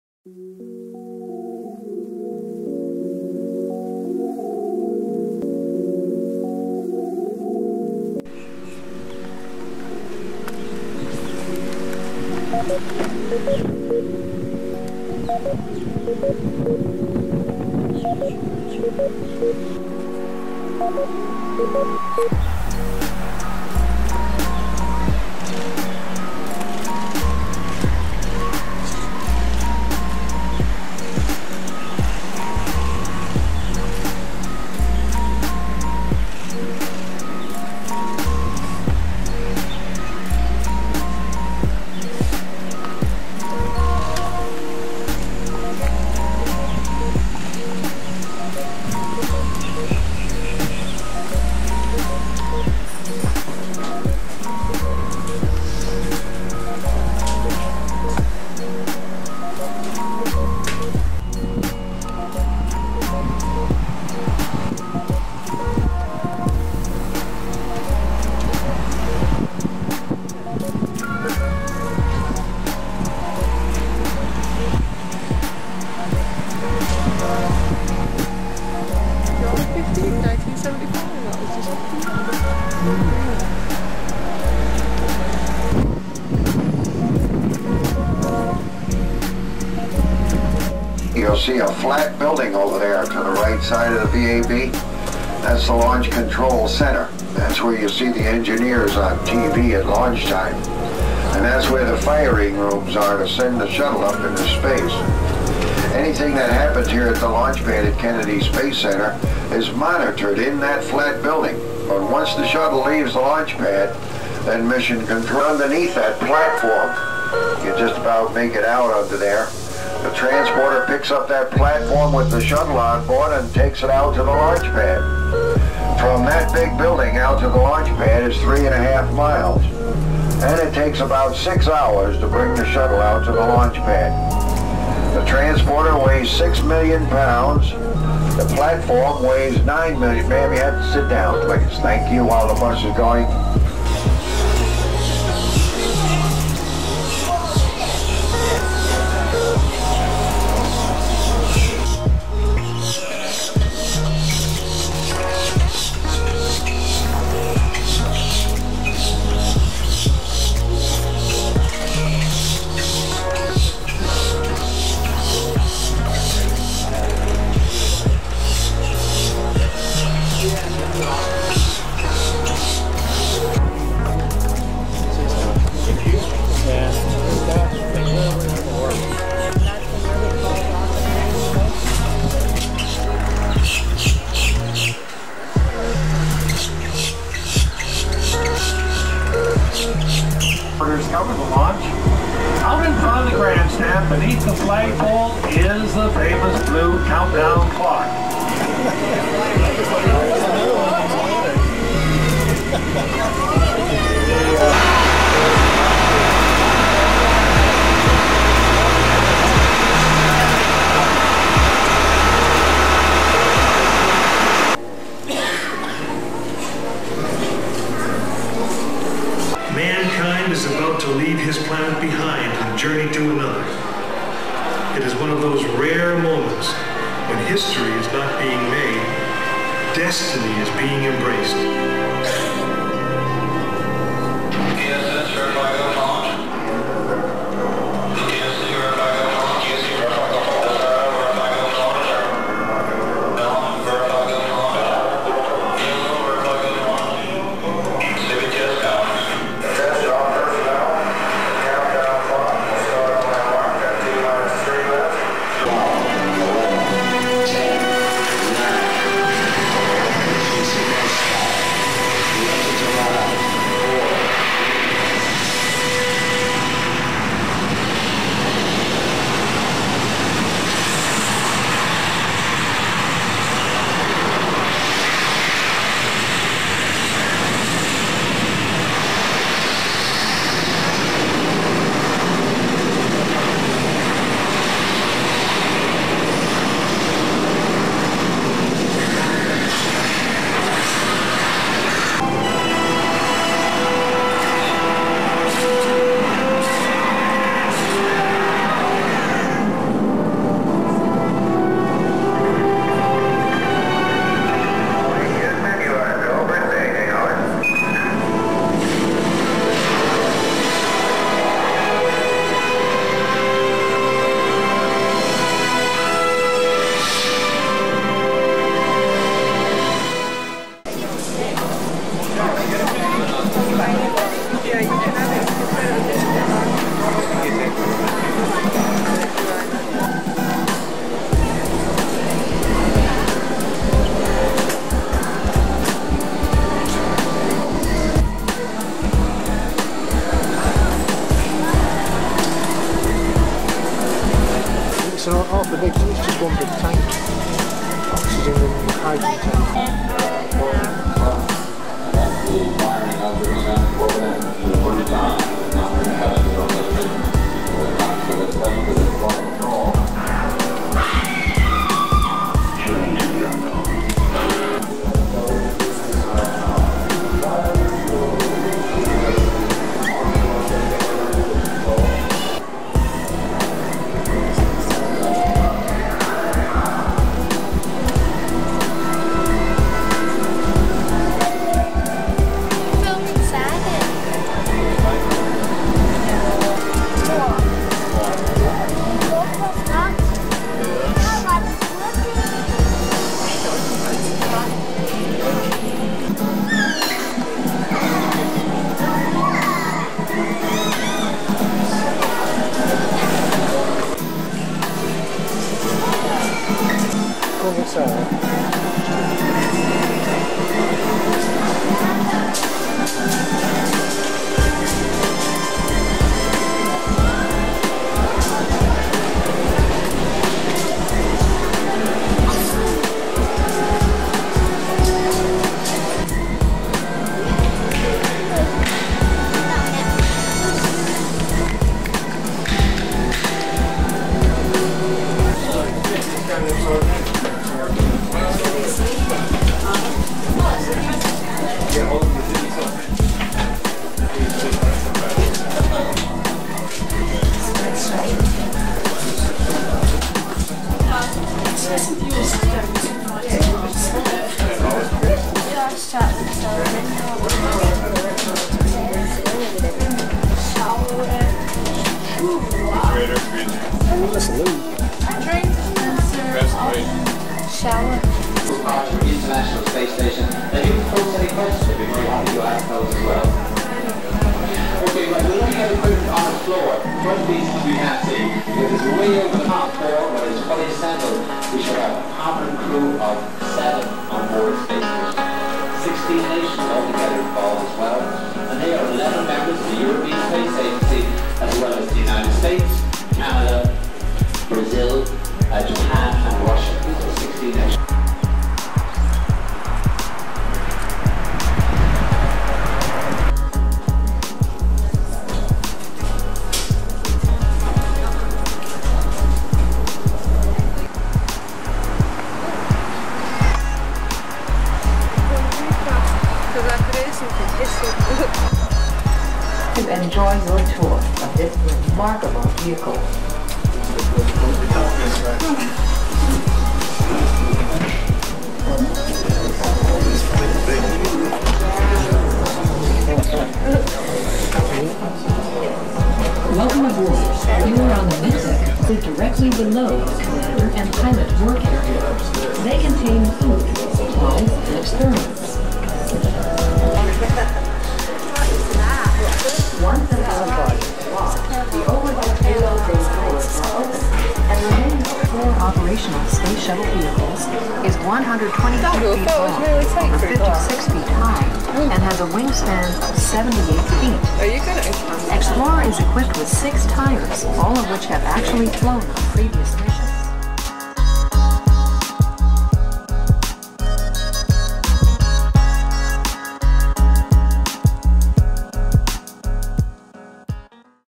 NASA November 1999 | Shuttle In Assembly Building | Shuttle On launch Pad
Welcome to our 1999 NASA trip.

During our Disney and Universal trips we took a day away from the parks and visited Cape Canaveral. On this trip was my Mum and Dad, my Sister and my Cousin.